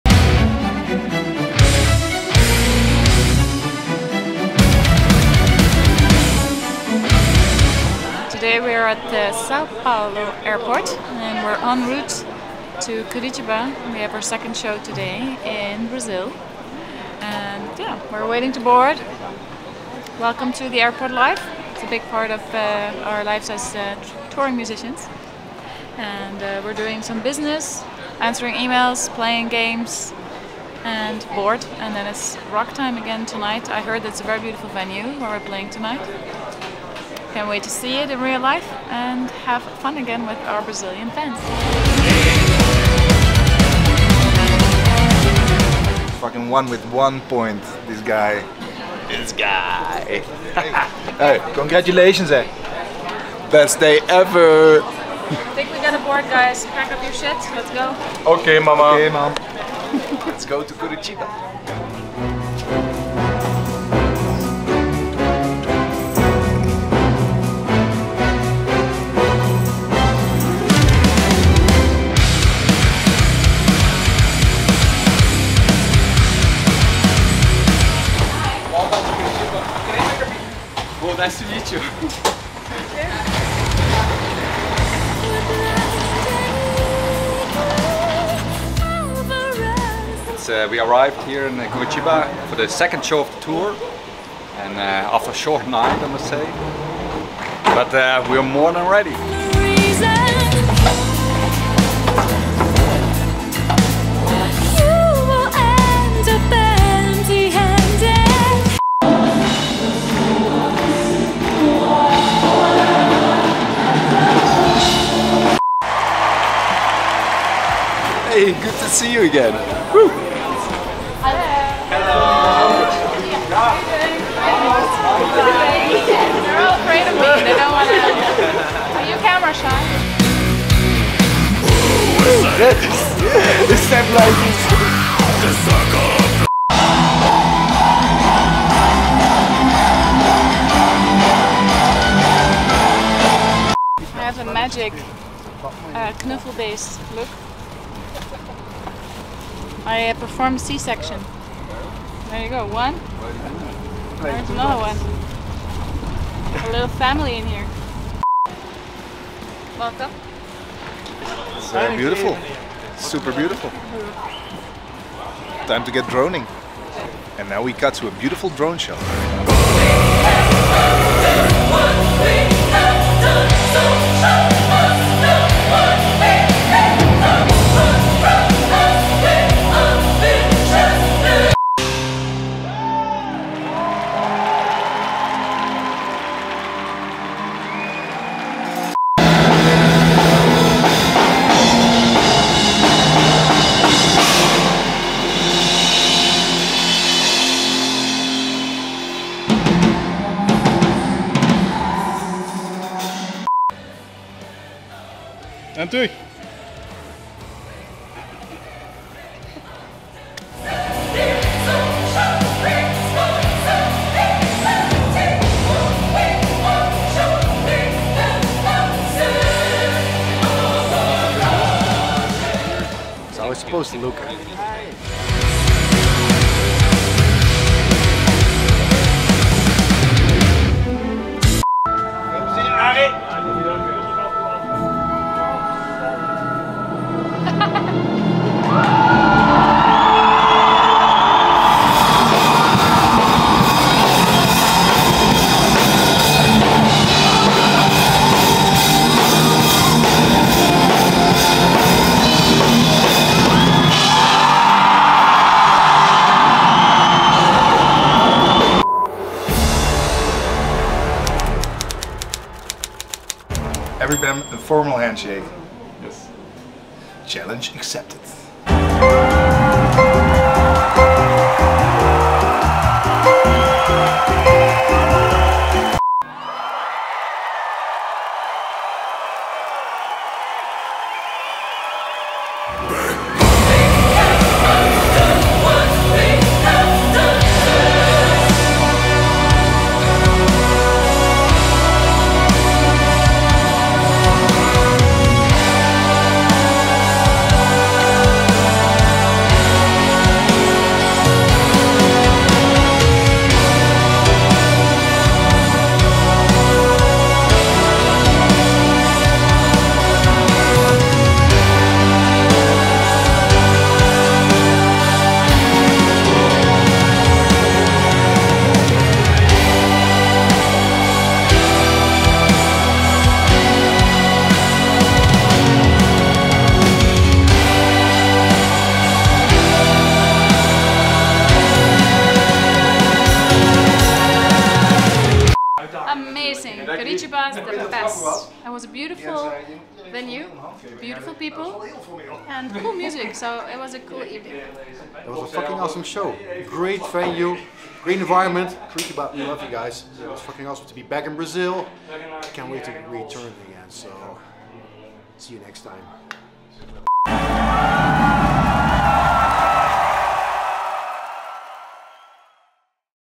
EPICA in Curitiba: Latin America Tour Vlog—Episode 2
On November 20, 2022, Epica returned to Curitiba, Brazil for the first time in nearly 4 years to play at Opera de Arame. Experience the day with the band as...
1️⃣ They mix business and pleasure at the airport (Coen vs. Isaac in a game of "Lost Cities") 🃏
2️⃣ Isaac deals with very intense turbulence during takeoff 😂
3️⃣ Simone shows off her magic knuffelbeest 🧸
4️⃣ Rob does a side-by-side comparison with CHIBI ROB! ✍️
5️⃣ Coen attempts the Challenge of the Day: to give every band member a formal handshake 🏆